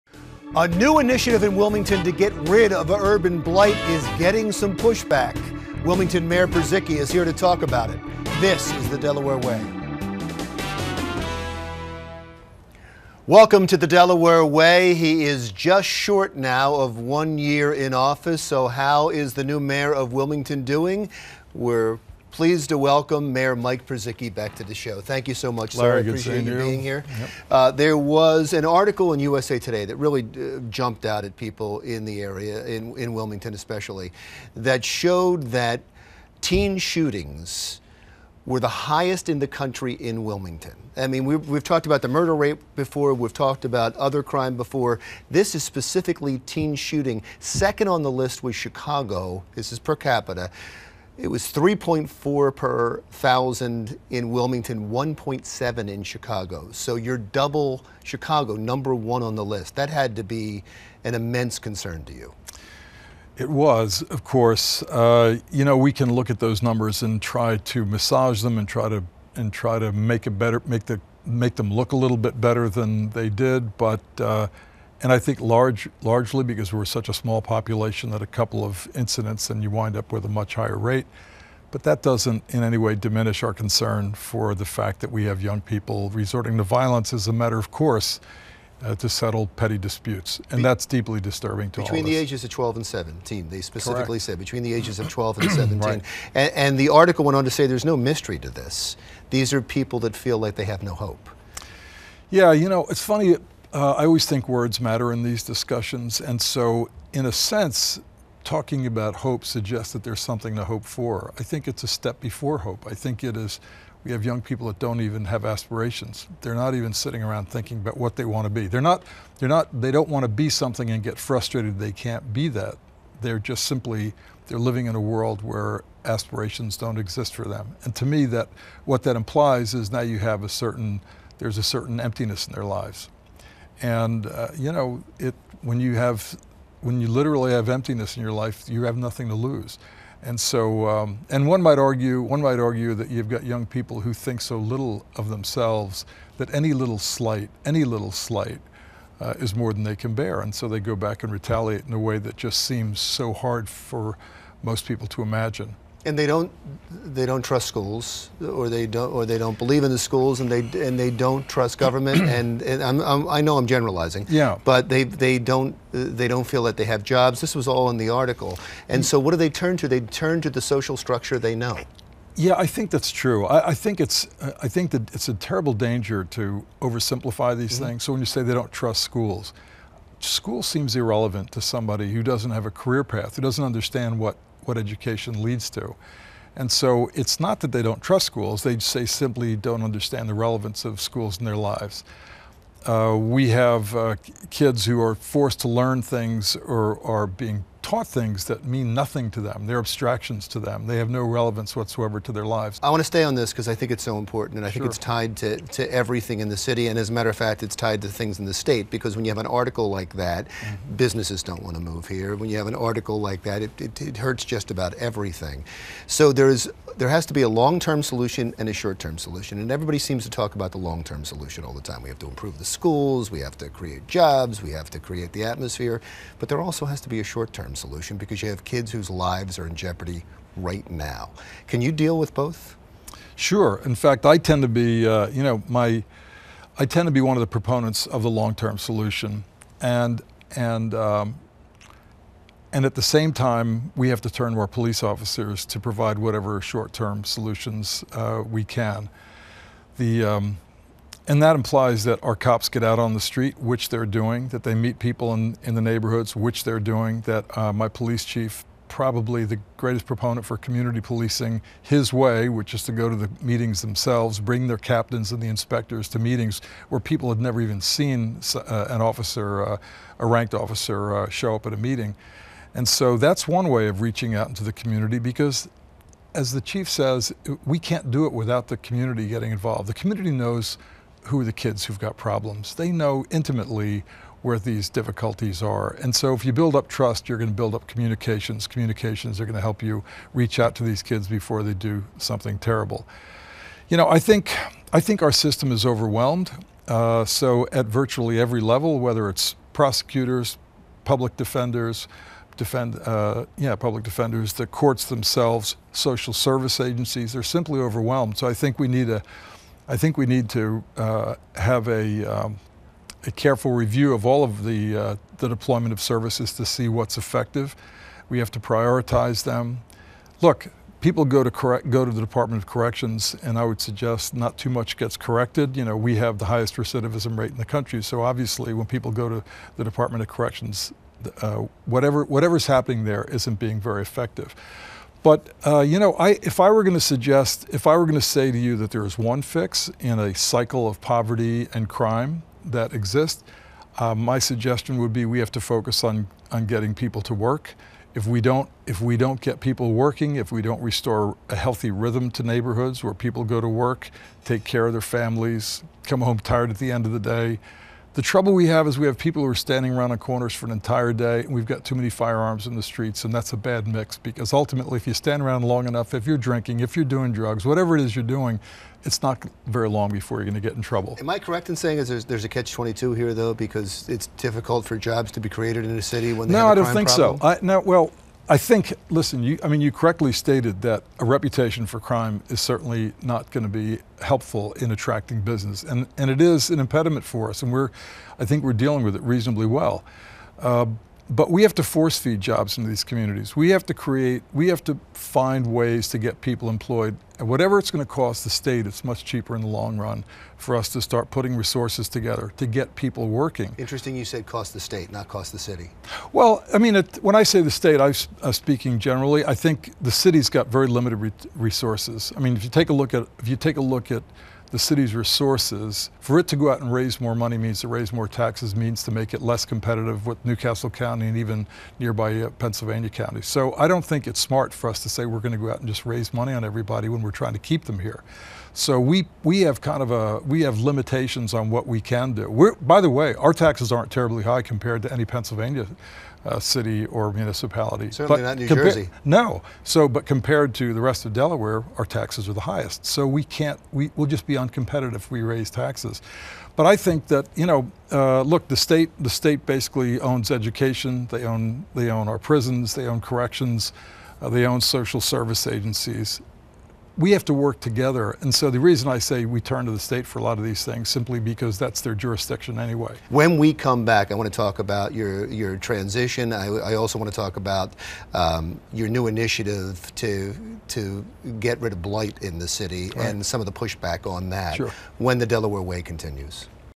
The Delaware Way - Mayor Mike Purzycki part 1
Larry sits with Wilmington Mayor Purzycki to talk all things, Delaware